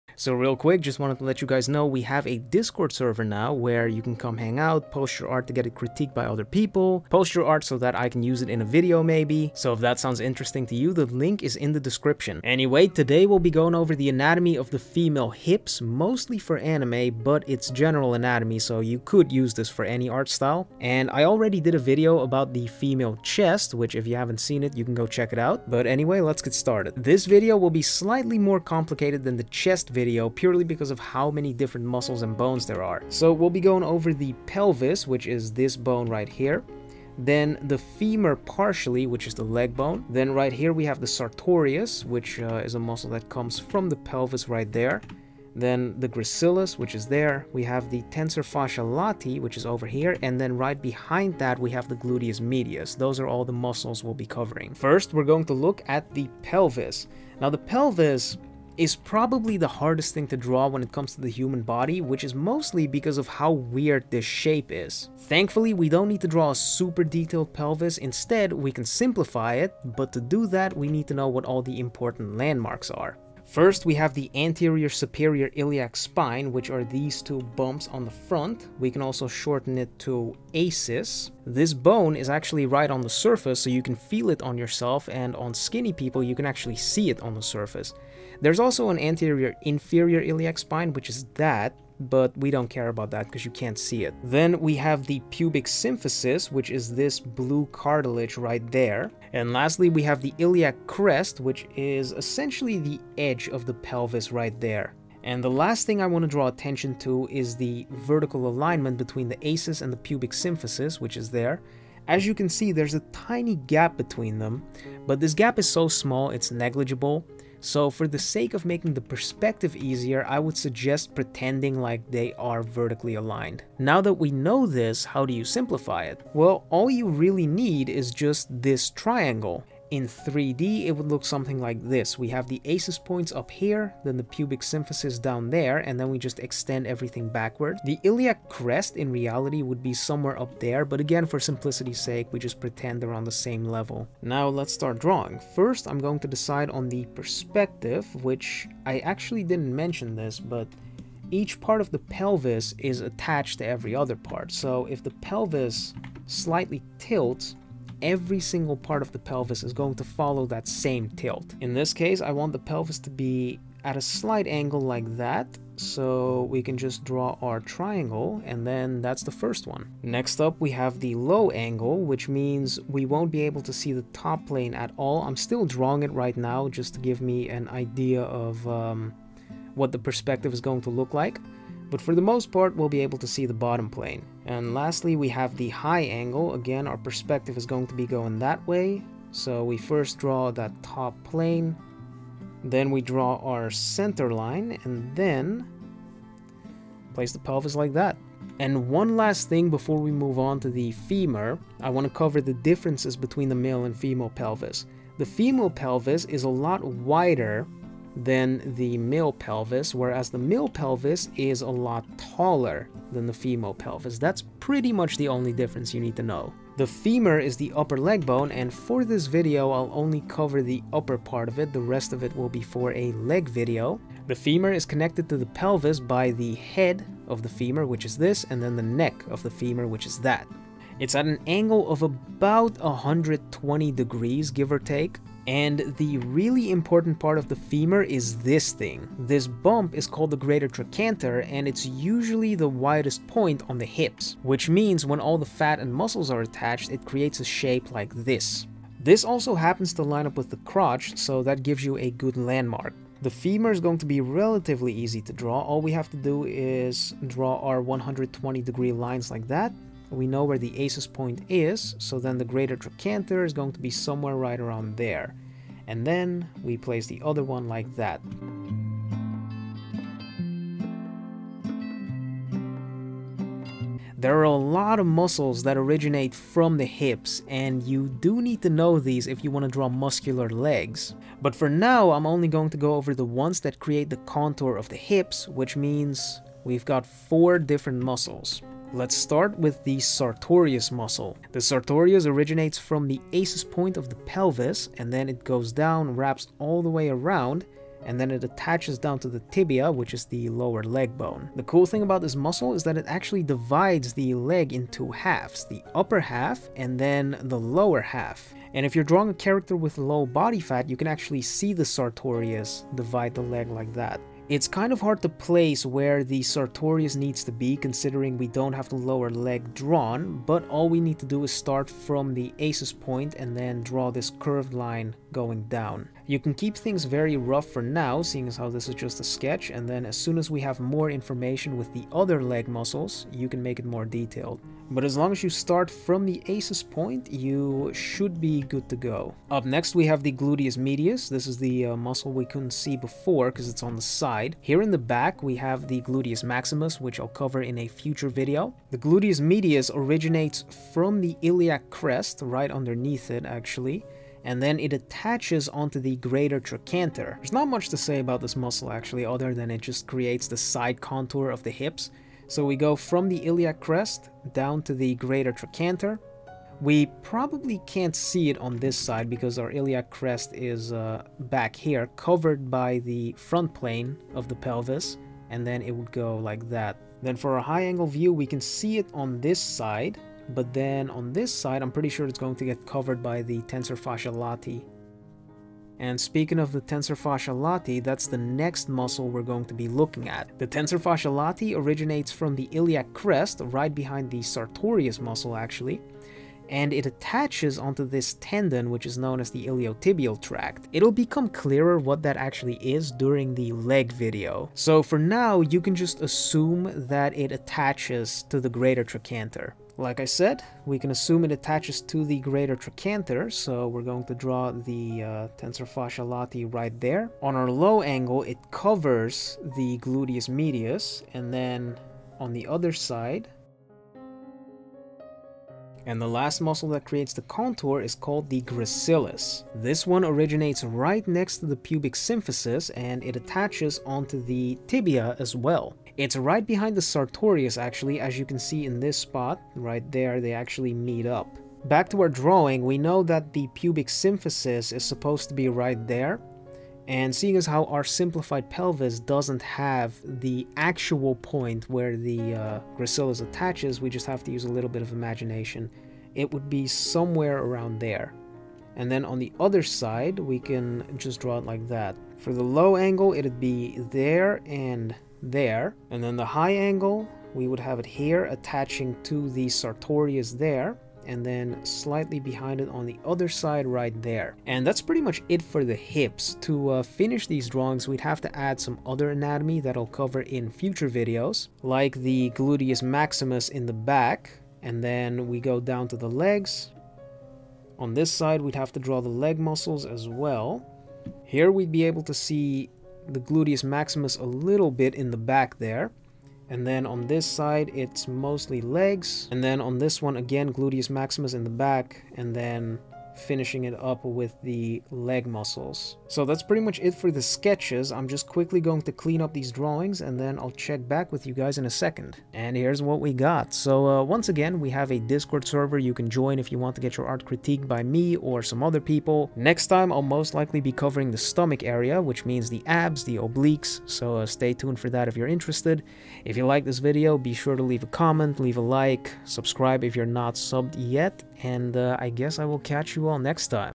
Anatomy of the Female Hips for Anime - How To Draw
In this video I'll be going over the anatomy of the female hips, mostly for anime characters, but this also applies to real people

Intro - 00:00
Hips - 00:27
Pelvis - 00:54
Femur - 03:36
Sartorius - 04:49
Gluteus Medius - 05:39
Tensor Fasciae Latae - 06:25
Gracilis - 07:06
Finishing Up - 08:32